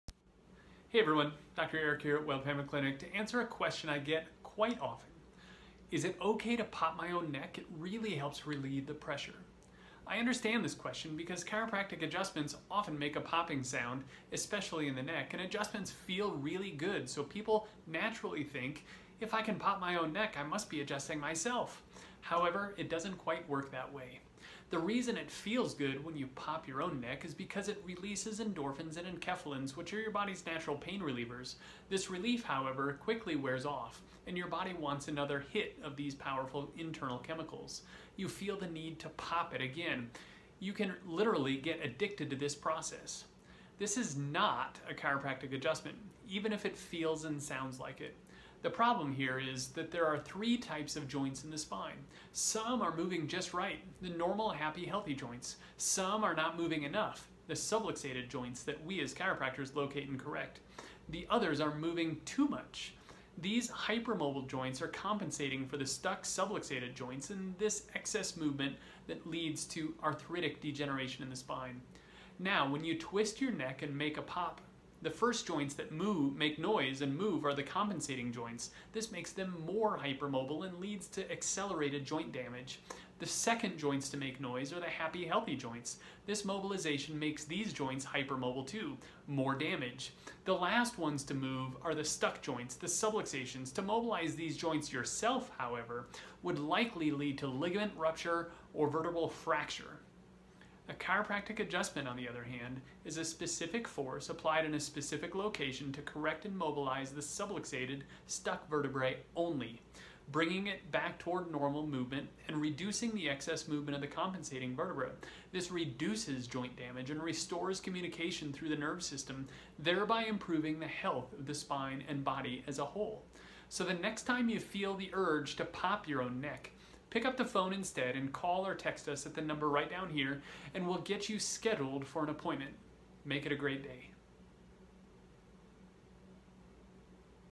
Is it OK to pop my own neck?
Even though it may feel and sound similar, popping your own neck is NOT a chiropractic adjustment! In fact, it will make things worse, not better.